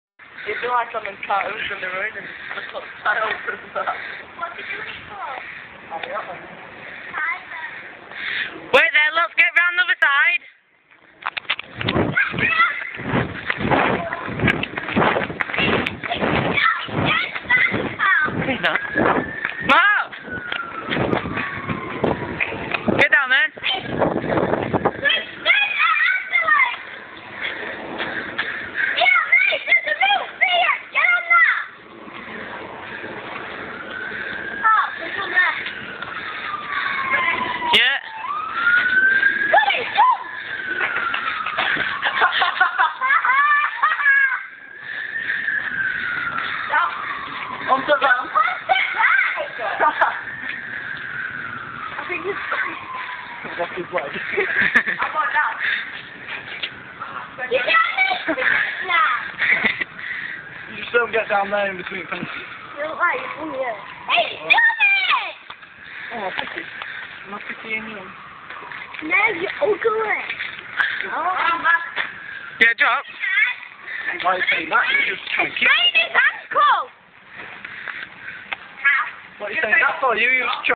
marks roof run
mark is on a mission. he's running away from the cops lol